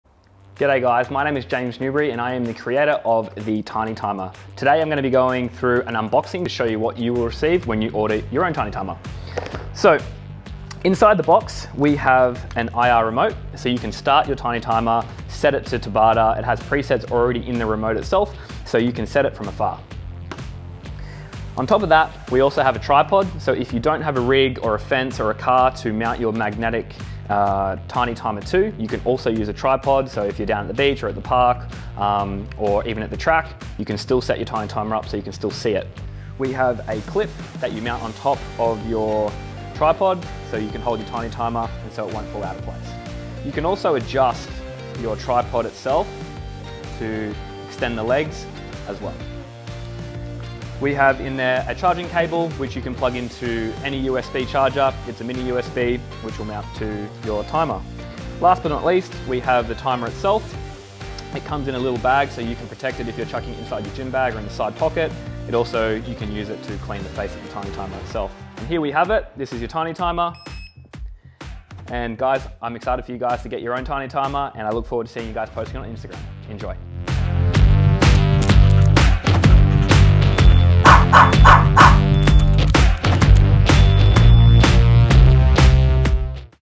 Unboxing The Tiny Timer with James Newbury
Developed by champion athlete James Newbury, the Tiny Timer is the most versatile personal timer you’ll ever have!

Ideal for workouts on the go, in the garage, at the track, park, beach or in your local gym.